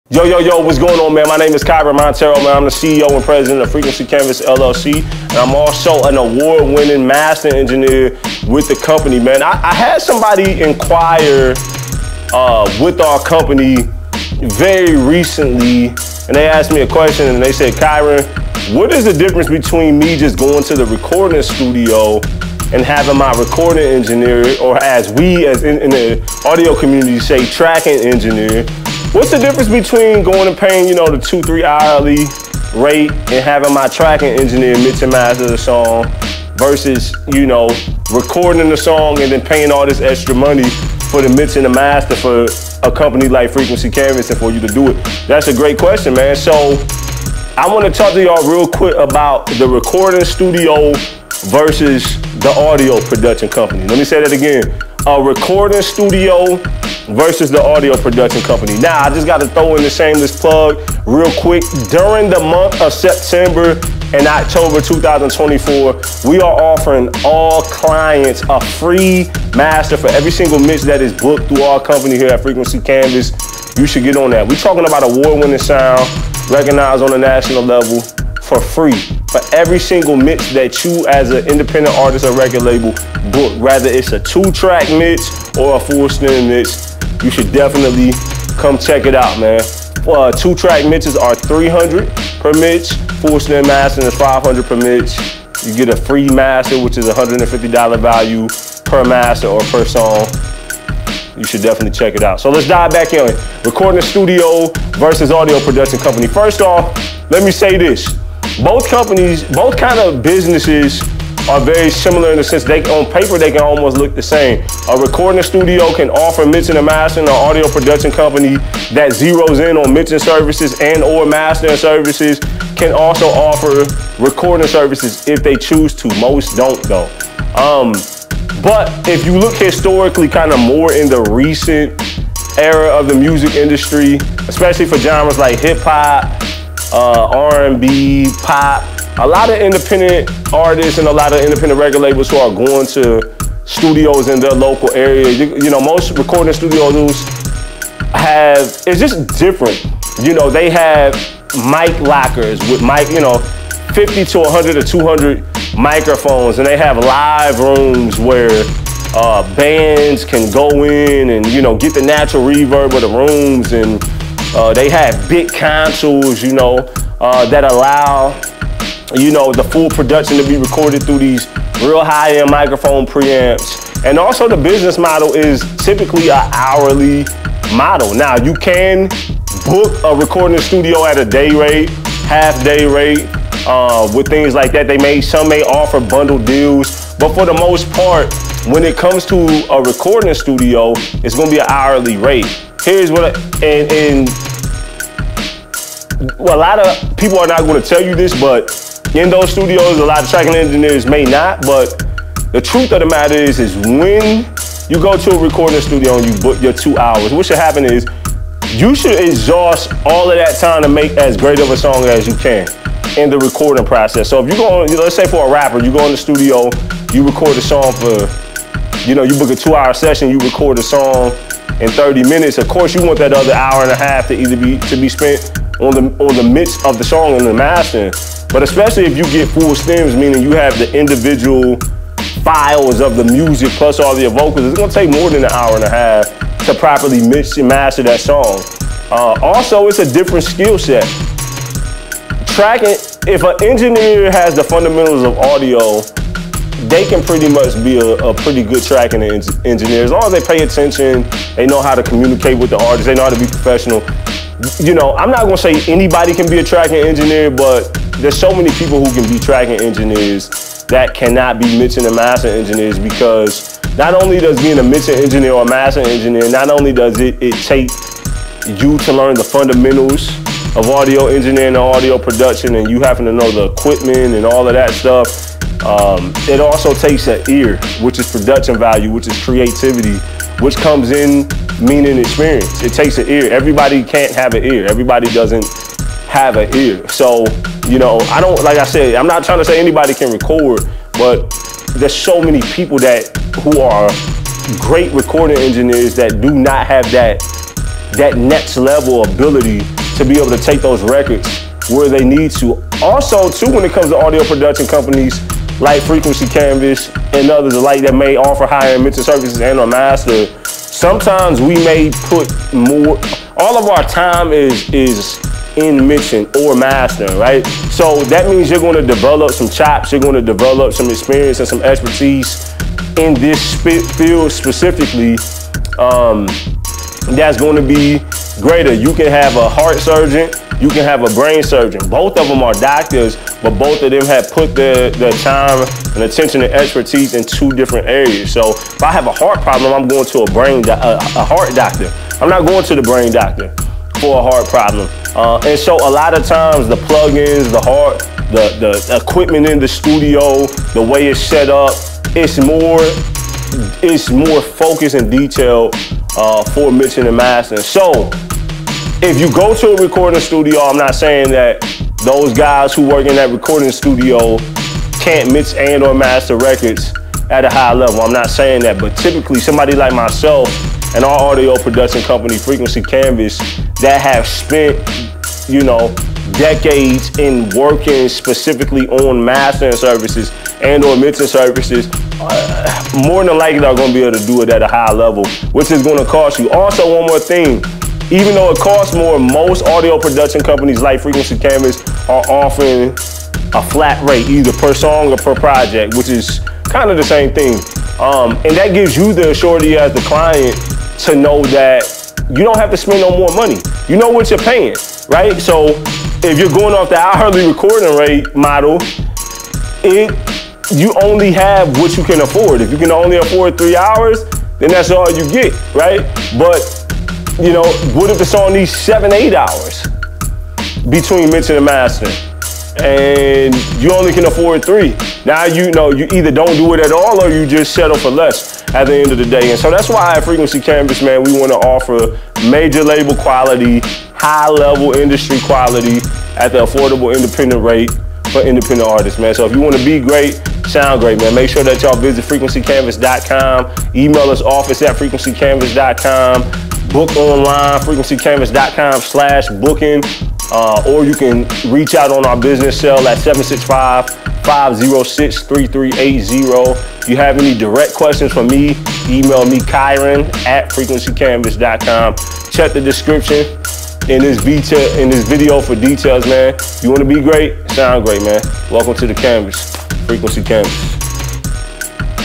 RECORDING STUDIO VS AUDIO PRODUCTION COMPANY W/ KYREN MONTEIRO
Let's dive in & talk about some the differences between Recording Studios who mainly specialize in recording services who offer mixing & or mastering services vs. Audio Production Companies who specialize in either mixing & or mastering services!

If you know someone you want to gift, then you can purchase a gift card on our